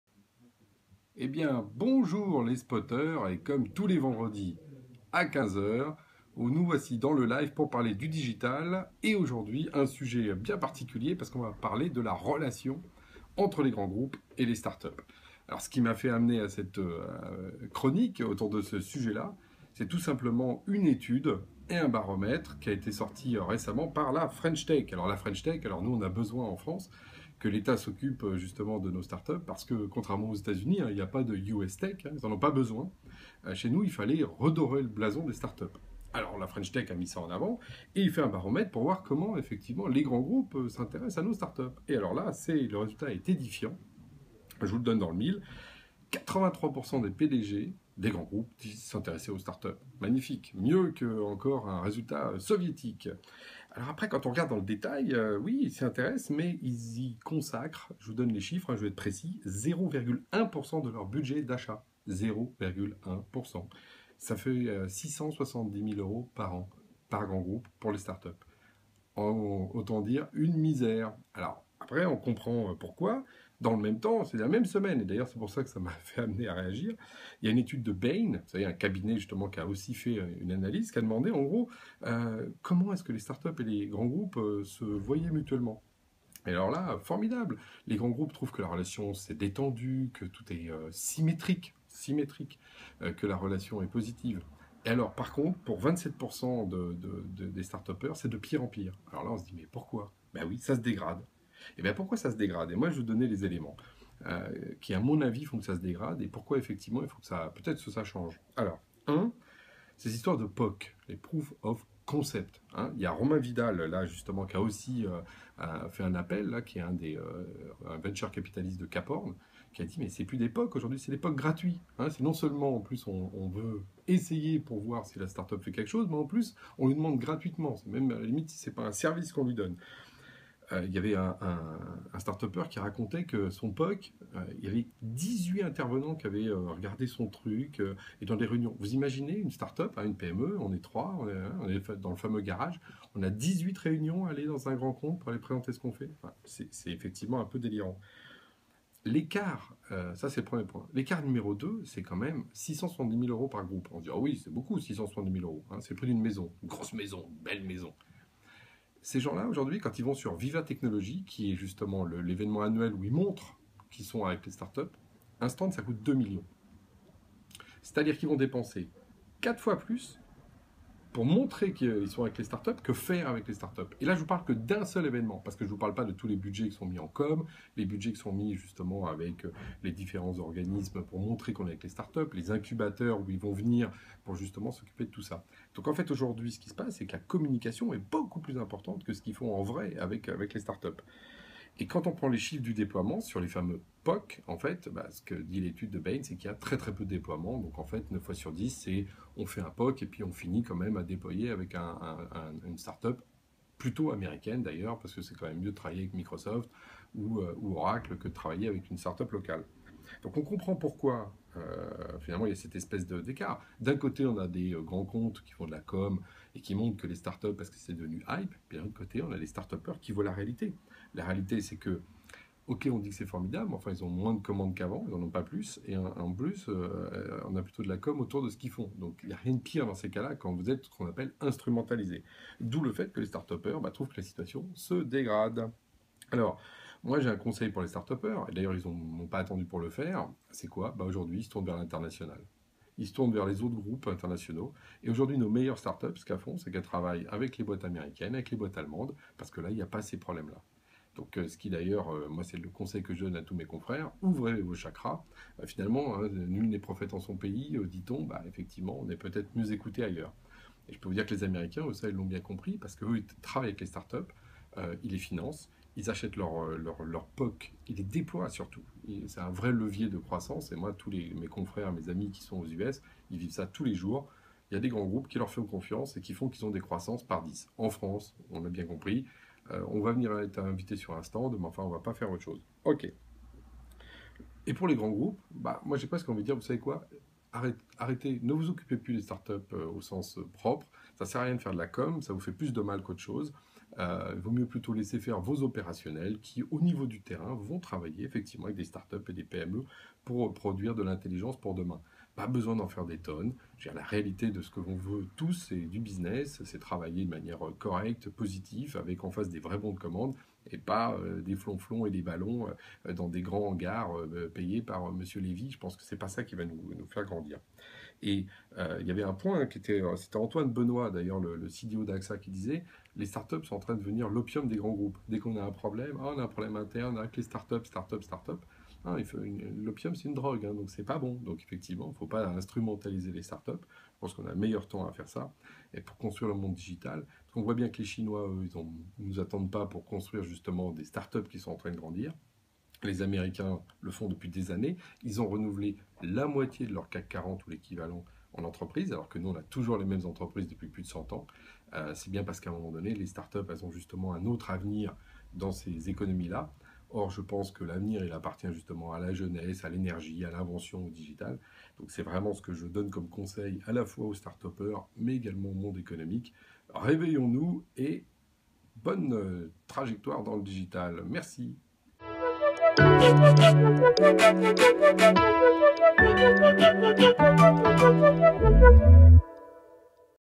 La relation grands comptes / startups
La relations grands comptes / startups : je t'aime - moi non plus

5 minutes pour un bref zoom sur les relations entre les grands comptes français et les startups.